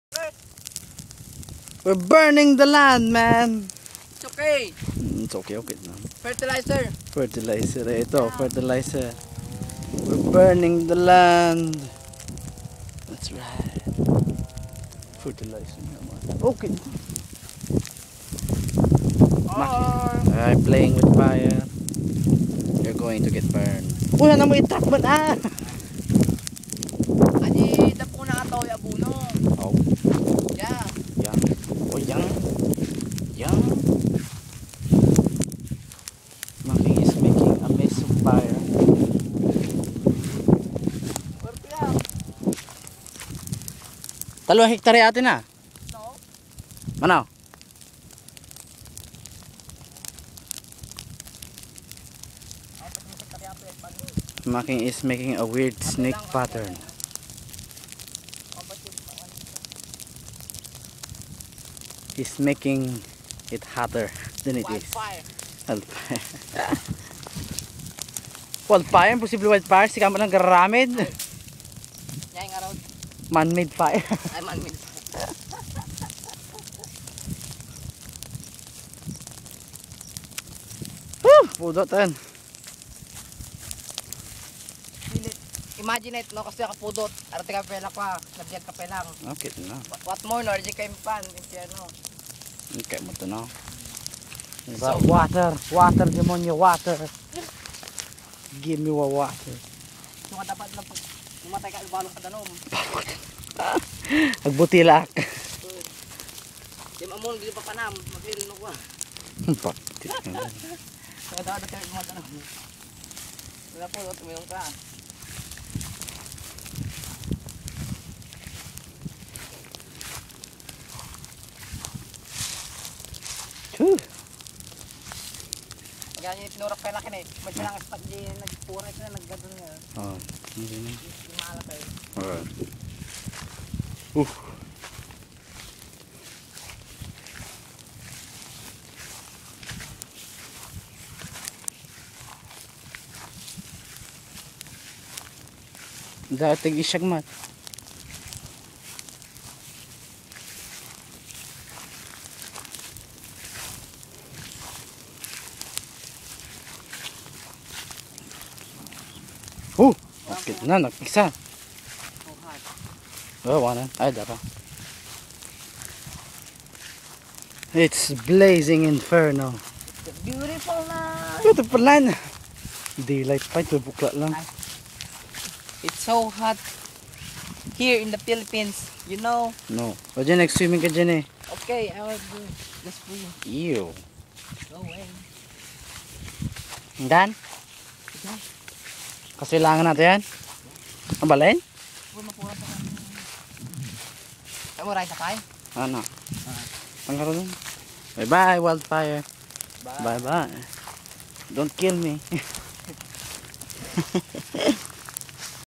Burning The Land Man
So my cousin and I are burning grass and straws to make fertilizer. It was high noon and we're making fire out in the searing sun. Good day, Good day...